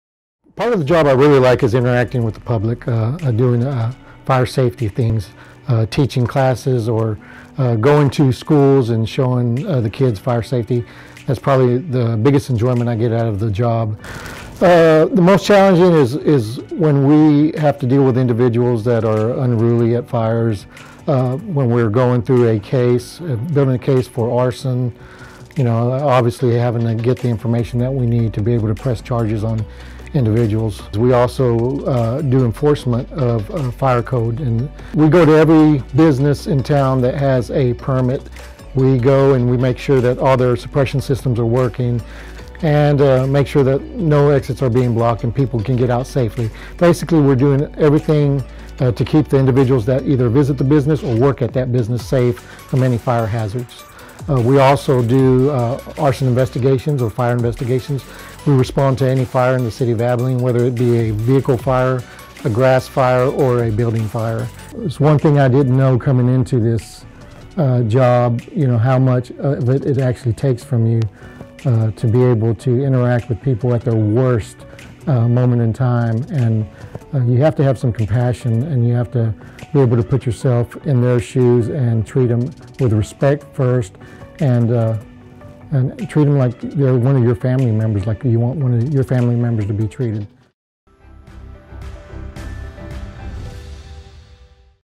Arson investigation & inspections with AFD's Richard Fuentes
Congratulations, Richard! Join us in congratulating Richard Fuentes as he marks 20 years of service with the Abilene Fire Department, and shows us some of what he does as an arson investigator and inspector.